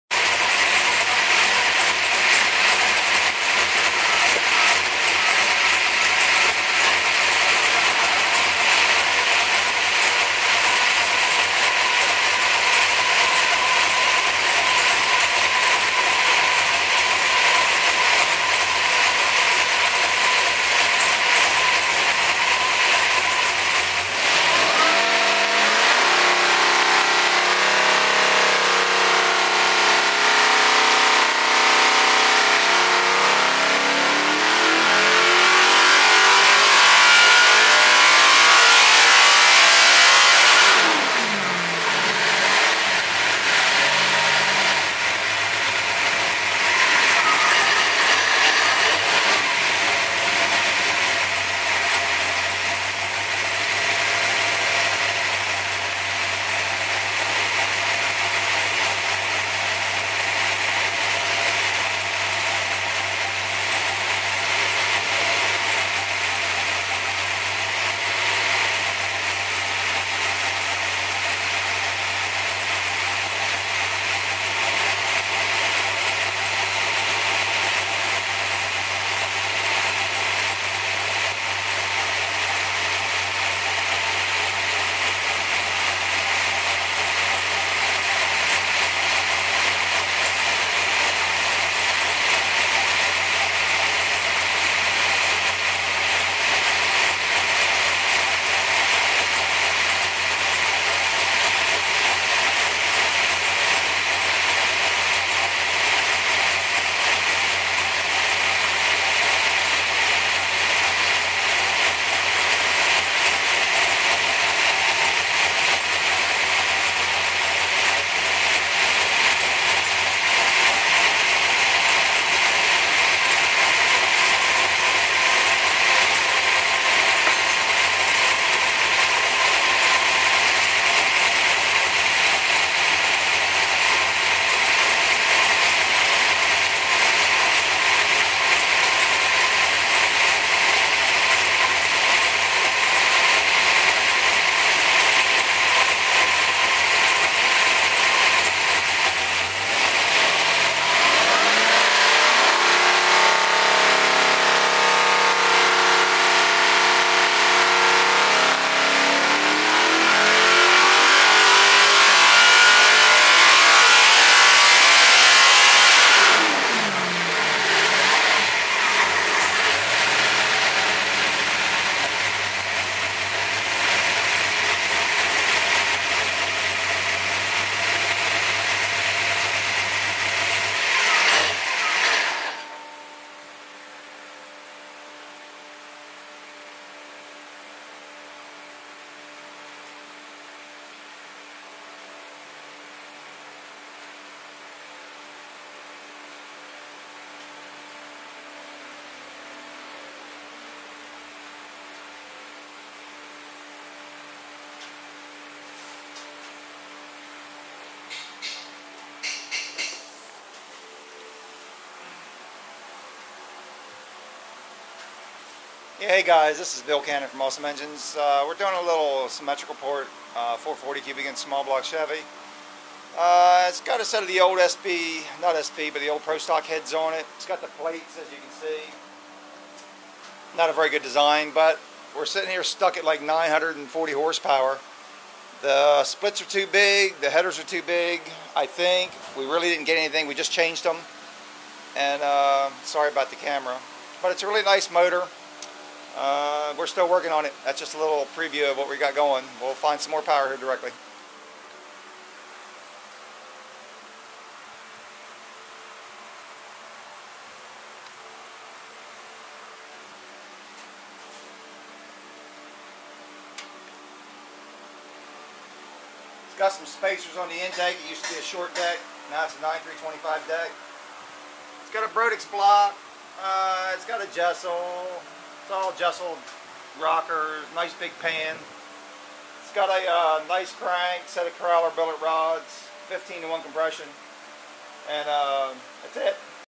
JAMES & CRYSTAL SMITH - 440ci SBC symmetrical port engine dyno pull
Used to be a 455ci 4.190 x 4.125 but the very short tires 28", 5 Speed, 4.10 gear put the rpm well over 7800 rpm. The aluminum MGP rods did not like that very much! We got a 4" Lunati crank, Crower Billet rods and JE 14.9:1 pistons all rebalanced and really thought we would have more upper end power. With the big 2.25/1.60 titanium valves and the massive Comp Special LS firing order, 7/16 x .165 wall push rods, Jesel rockers, belt drive and the completely rebuilt 5 stage Moroso Dry sump system we thought we would be banging 975/1000hp. The sheet metal with the Davanci splits and the killer linkage system would really pop. We set the front Mallory Drive distributor to 30° with 114 Dragon Race fuel and the engine done 929hp @  7400 so we gave it 2 degrees cam timing and the engine liked it but only done 939 @7150. It picked up overall but just lower the peak 350 rpm. The carbs were going rich in the upper rpm band but James wants to run the Q16 and see what happens. That should pull the right amount of fuel out to make the 975 happen along with 150+mph air coming in we can not pull in our dyno room. The engine is very smooth with absolutely no problems. I would put injection on this thing quick and know it would pull over 1000hp. Did not make any real power until 5800 rpm and did have a wide power band all the way to 7200. The one thing we do here at AwesomeEngines® is use a 50lb flywheel on the engine, plus a 20lb drive shaft to connect the engine to the dyno, then a 10lb aluminum flywheel to start the engine with and ofcourse the 60+lb of rotational weight really hurts the hp. But, the dyno is deadly accurate.